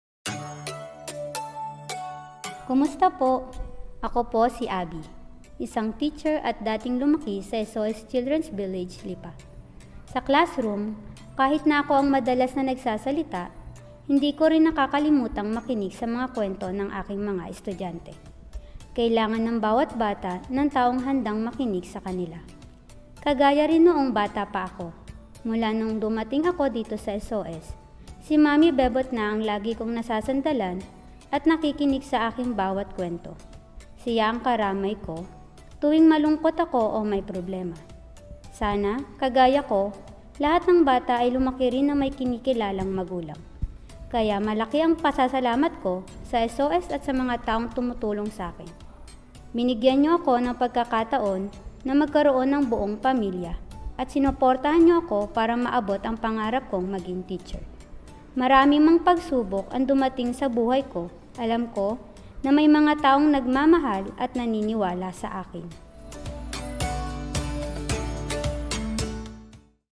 Abby's Success as a Teacher: A Testament to SOS Children’s Villages' Impact
It takes a good amount of strength to face life's challenges, and it's easier to be strong when the people you love support you. Abby was able to face her life with her head held high because of the people in the village who loved and supported her.